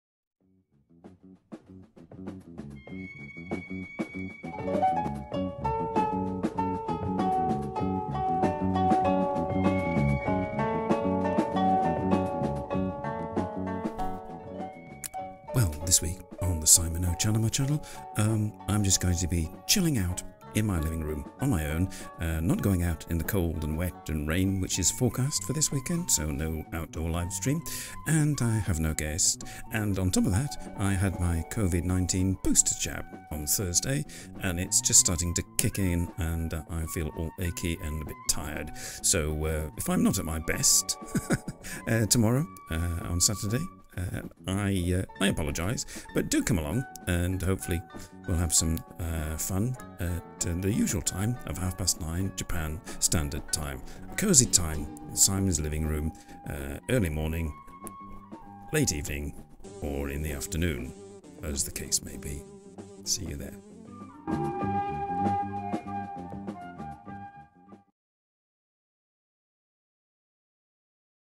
Simon NoChaNoMa in Post Booster Blues&Chill
Return to the Livingroom this saturday as i rest from the effects of my covid booster jab. So no outdoor livestream (to cold and rainy anyway) and also no guest this week. Feel free to come and chill with me at your usual viewing time. Hopefully see you there :)